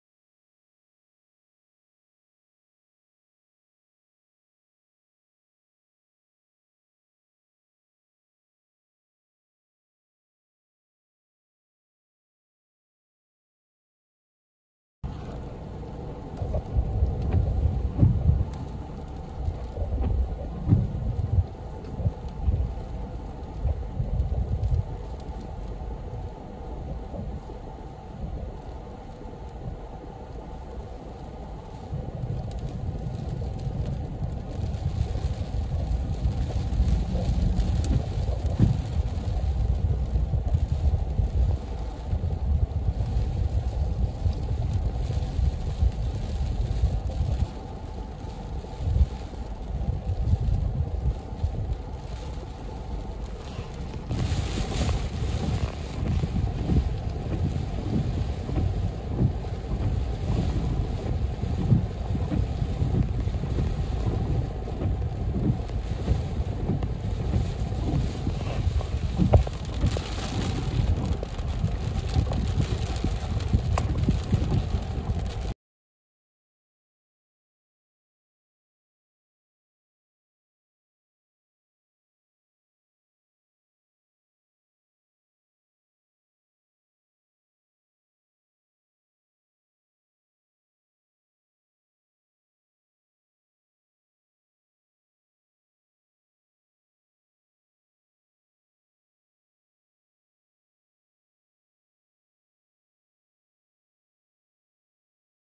07-29-2023- Bedford County Severe Thunderstorm with Damage in Stewartsville and Thaxton VA
A strong thunderstorm caused damage in Bedford County VA east of Roanoke VA on 7-29. Trees sheds and other items were thrown and damaged .